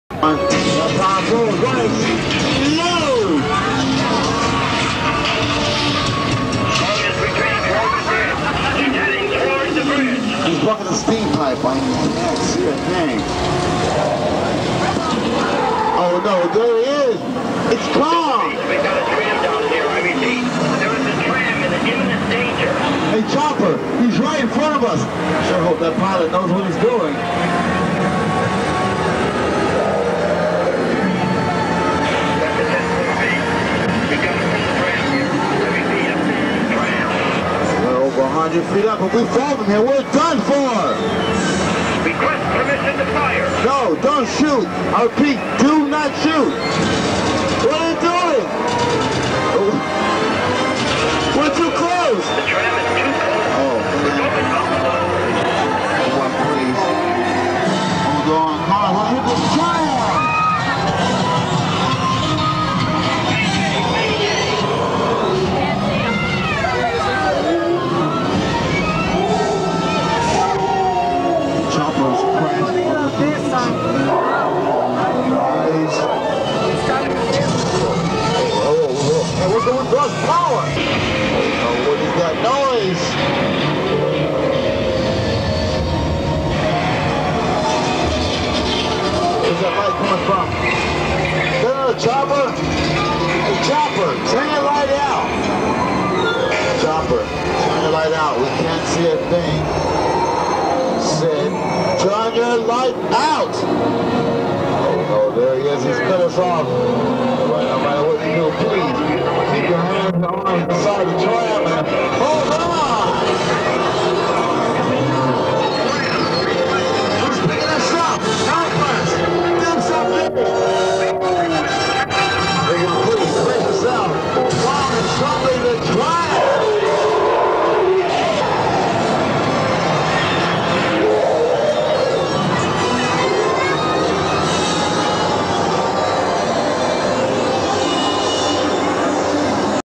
King Kong Ride Universal Studios
King Kong Ride at Universal Studios, Orlando Florida shot by my father with an VHS camera in 1997.  Couple shots are dark and cut some of them out but some great video of the ride and will bring back some nostalgic  memories for anyone that ever rode it! Enjoy